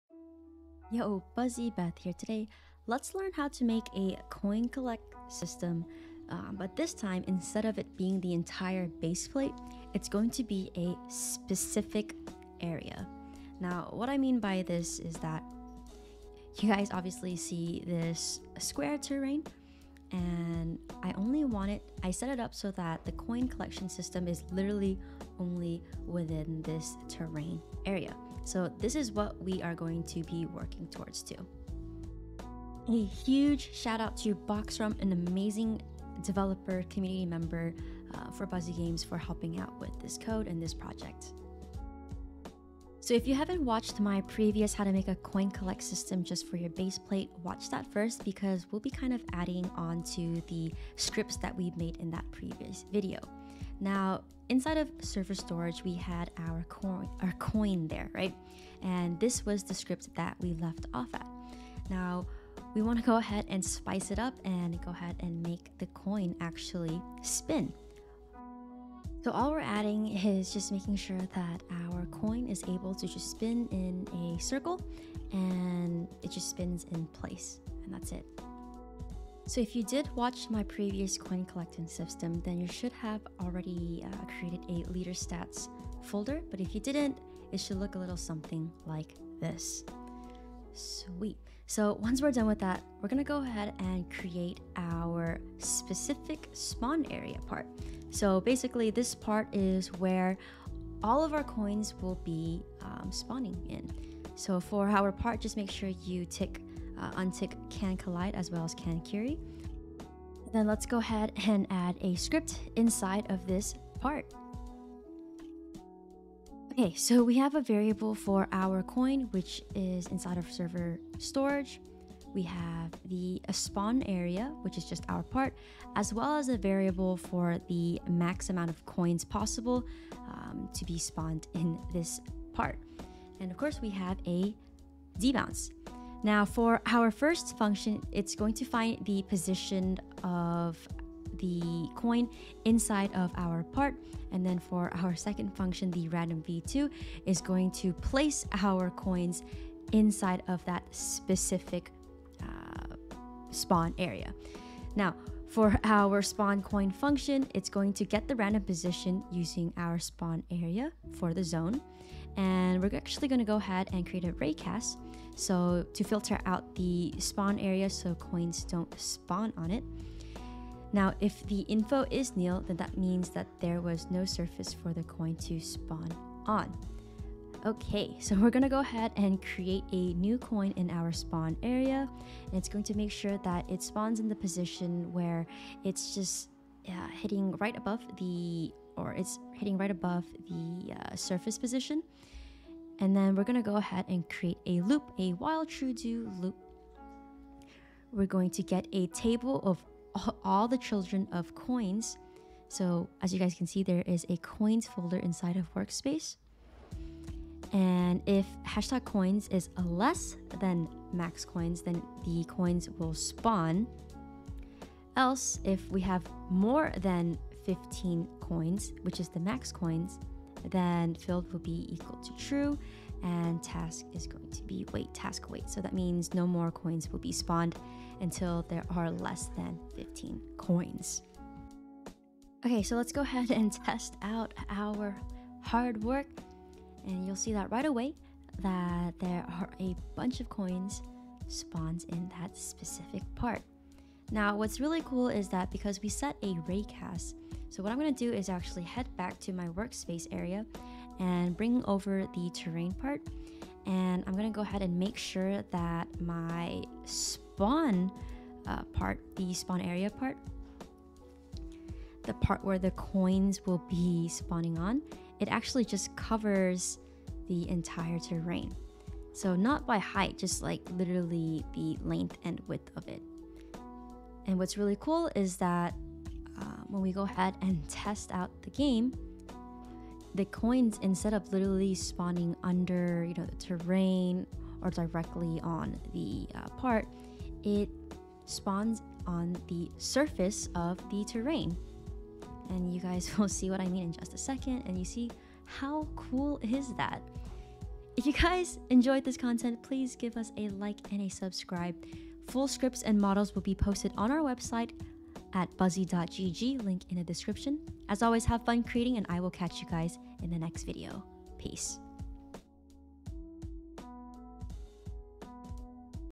How to Make a Coin Collect System in a Specific Area | Roblox Studio
In this video we'll be learning how to create a coin collection system in a specific area (part)!

Watch this coin collection system video first before watching this one so you don't get confused